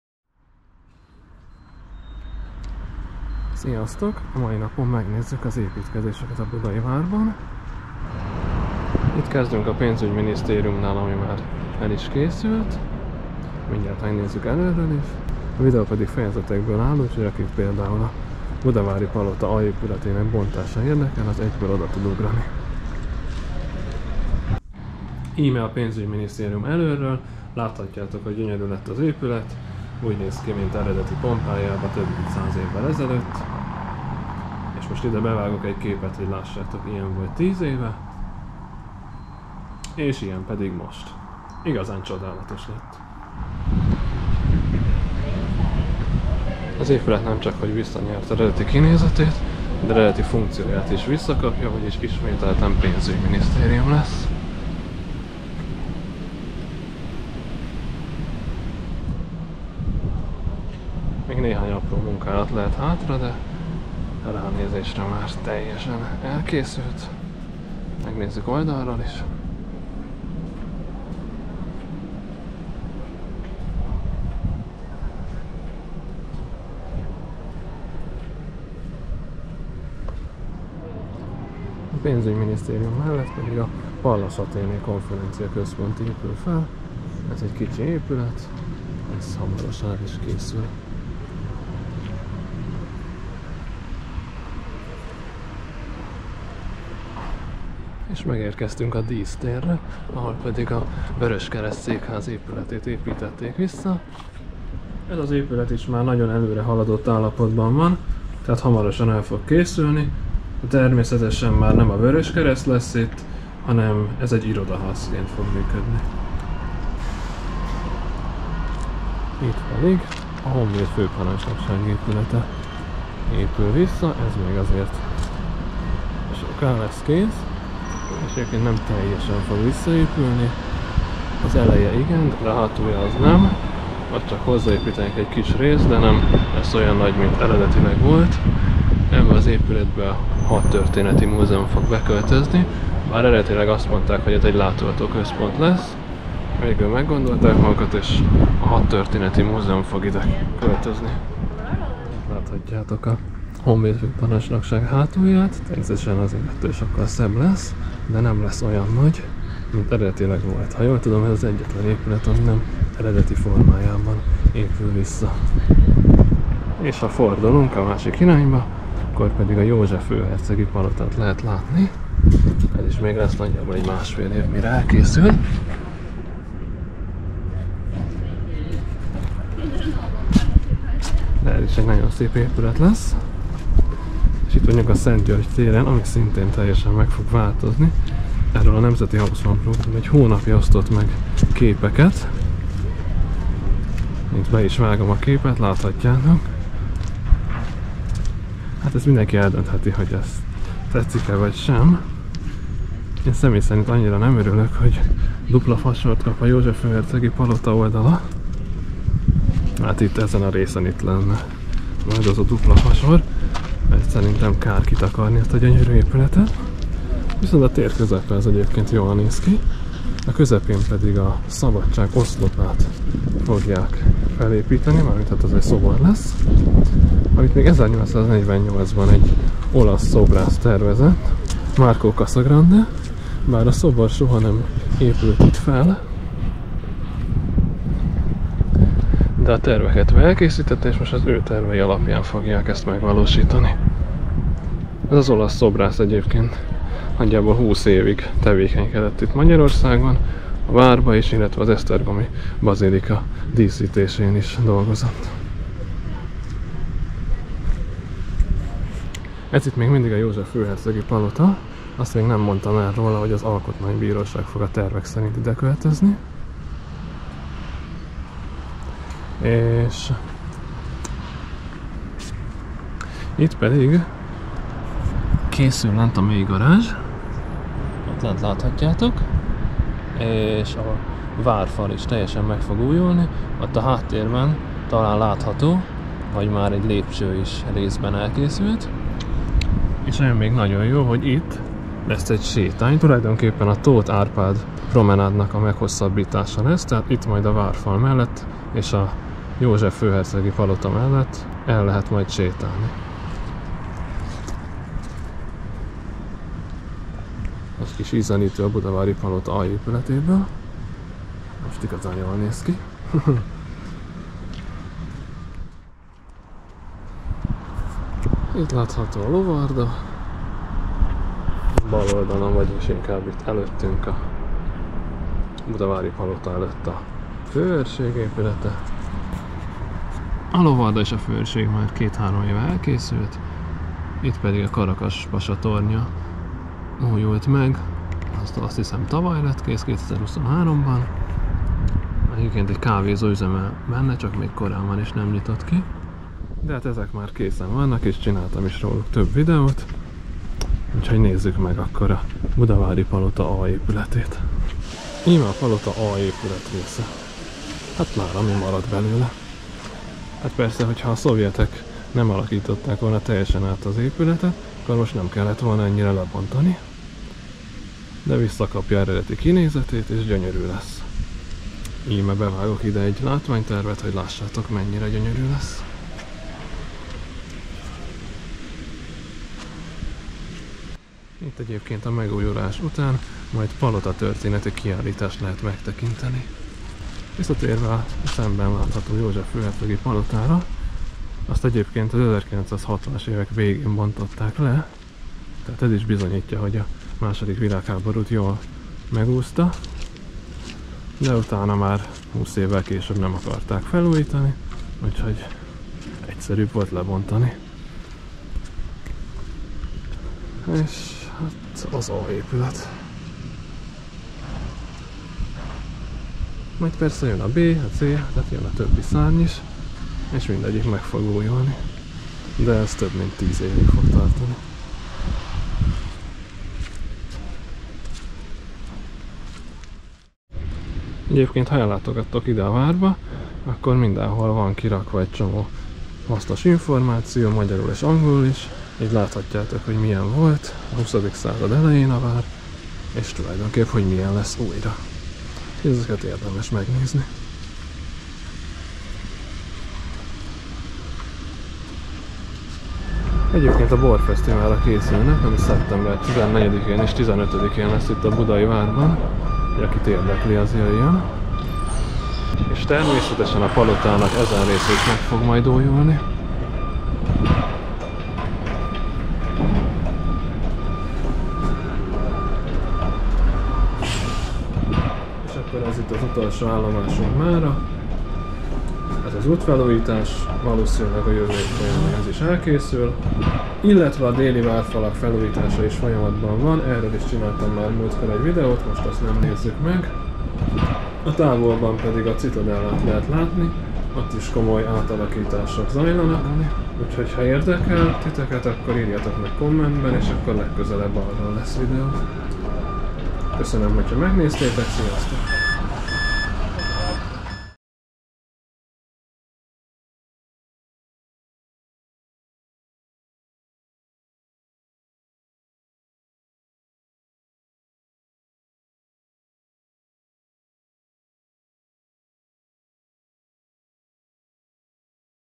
Budai Vár építkezések - 2024. szeptember
Köszönöm, ha támogatod a munkámat!

"Ilyen lesz" látványtervek a Nemzeti Hauszmann Program képei!

00:00 - Bevezető
00:25 - Pénzügyminisztréium
01:22 - Pallas Athene Konferencia Központ
01:37 - Vöröskereszt Székház
02:00 - Honvéd Főparancsnokság
02:47 - József Főhercegi Palota és Szent György tér
04:35 - Mélygarázs, Nyugati Várfal
05:17 - Lovarda, Főörség
05:55 - Budavári Palota A épület
07:57 - Útfelújítás